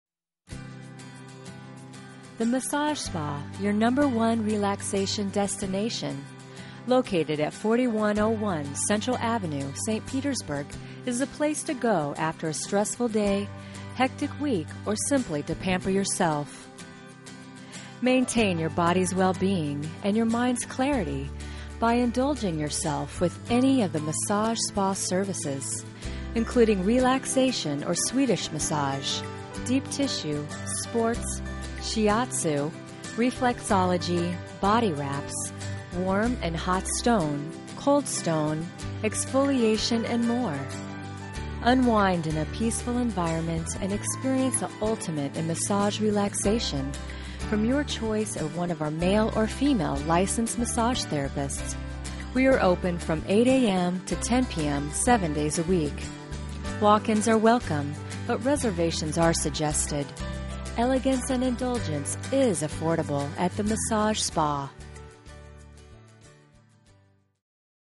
The Massage Spa
Come take a tour and get a massage at the elegant & affordable The Massage Spa in St. Petersburg, Florida